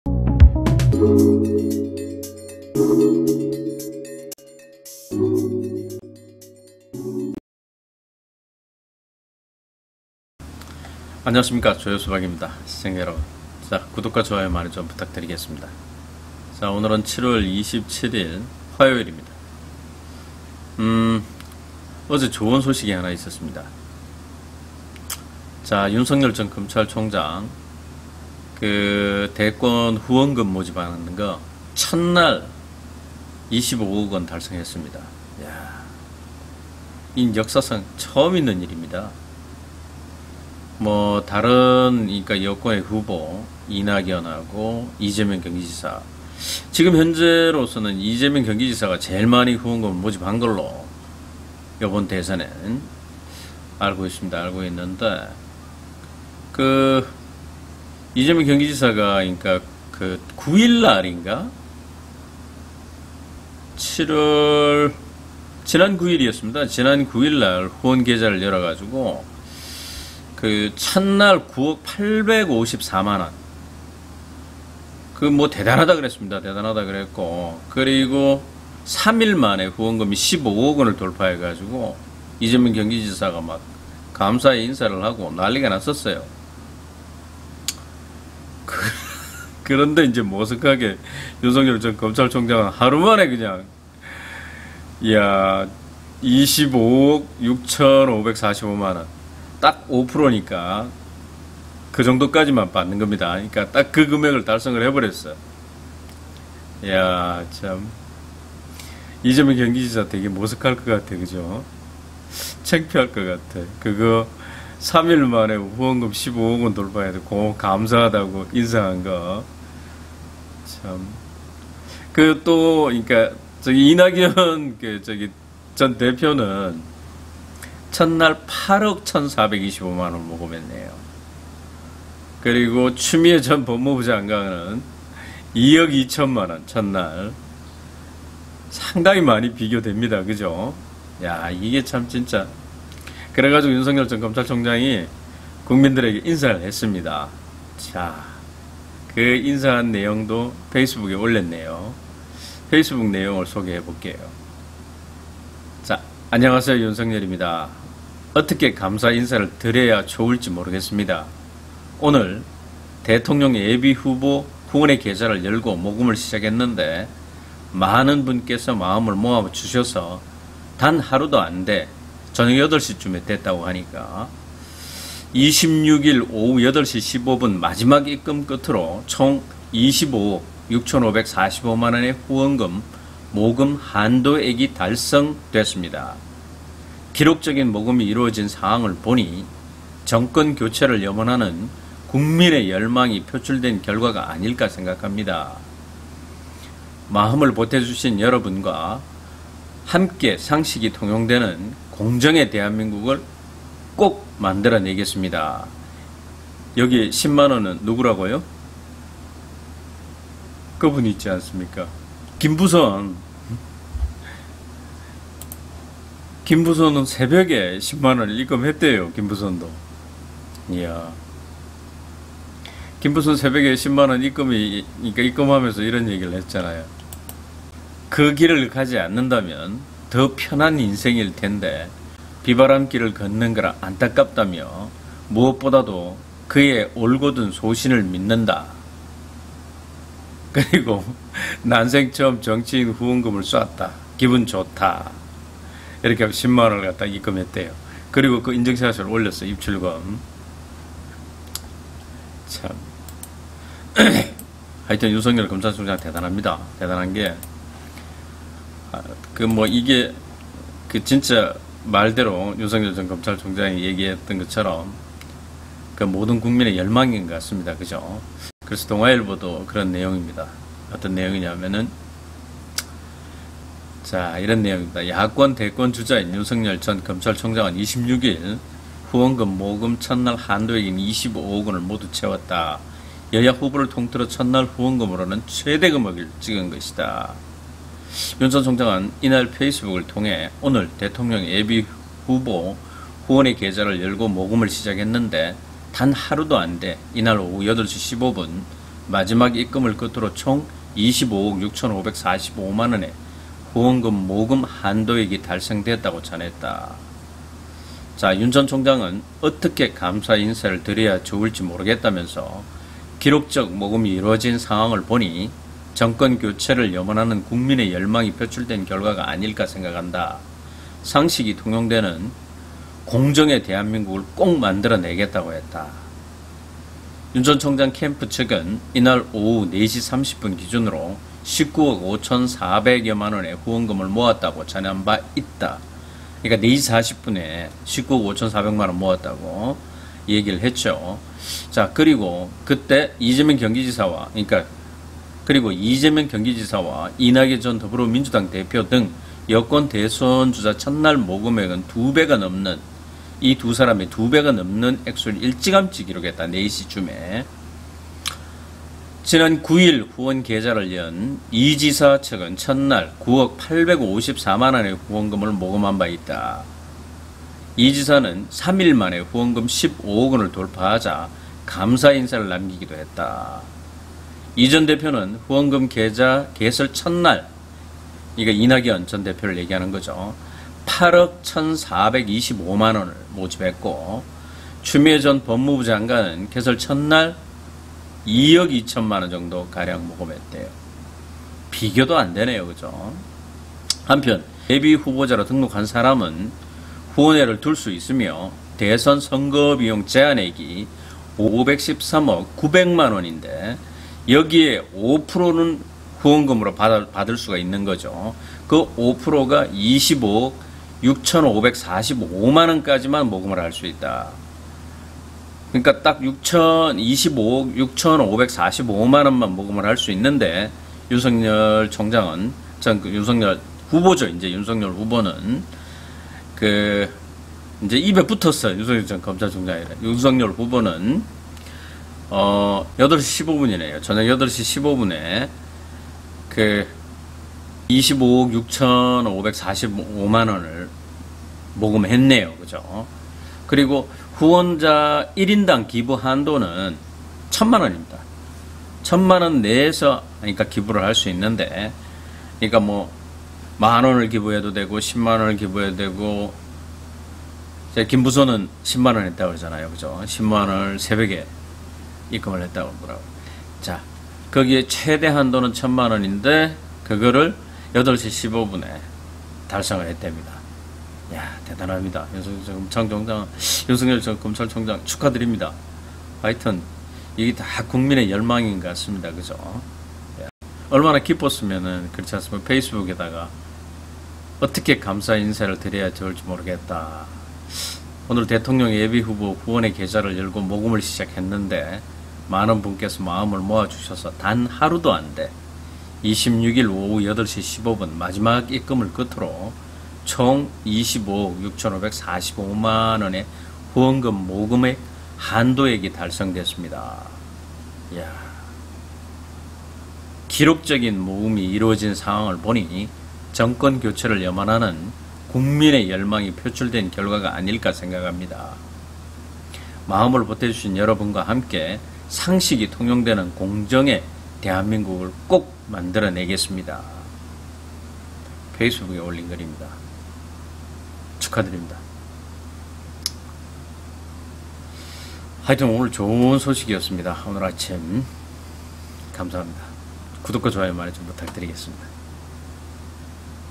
윤석열 후원금 25억 최단시간 후원자 90% 이상이 소액 후원#국민의열망
윤석열 후원금 25억 최단시간 윤석열 후원금 25억 최단시간 후원자 90% 이상이 소액 후원 공정과 상식의 세상을 바라는 국민의 뜨거운 열망